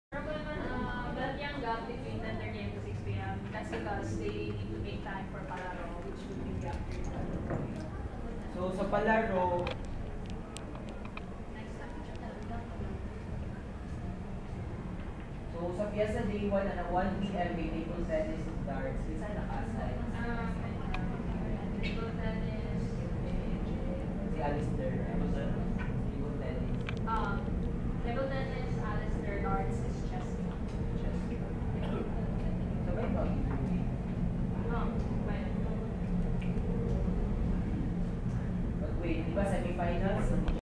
Staff members listening
WITHIN ATENEWS: FIESTA GENERAL ASSEMBLY
Staff members attentively take note of the actions and the reminders from the heads.